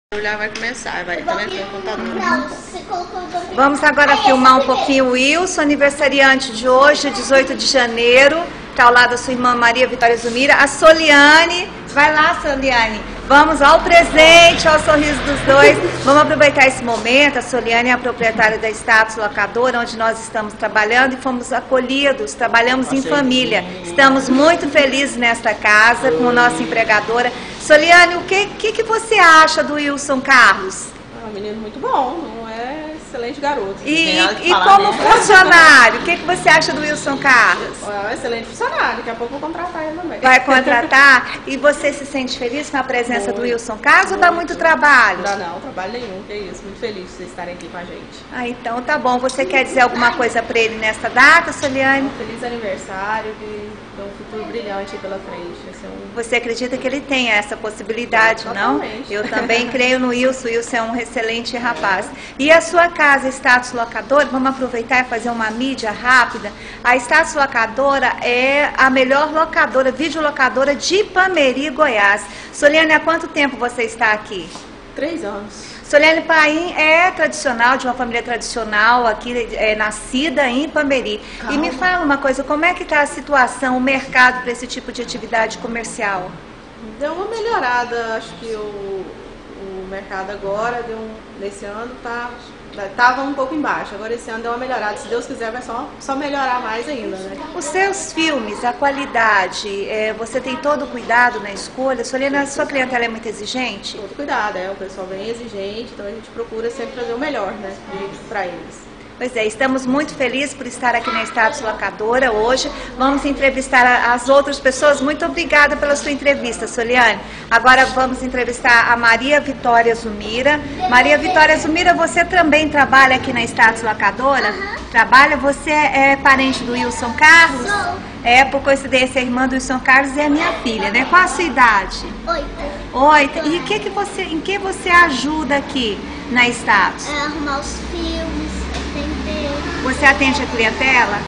ANIVERSÁRIO DO WILSON CARLOS EM 18/01/2012, OUÇAM O DEPOIMENTO DA NOSSA EX-EMPREGADORA SOBRE ELE
Obrigada! Naly de Araújo Leite

Família
Sempre trabalhei tendo os meus filhos comigo, e eles trabalhando e ajudando em todas as funções, após as aulas, até final do período de trabalho.
Dificuldades ou não, eles sempre foram meus filhos e companheiros na caminhada.
Grata a Deus!
Naly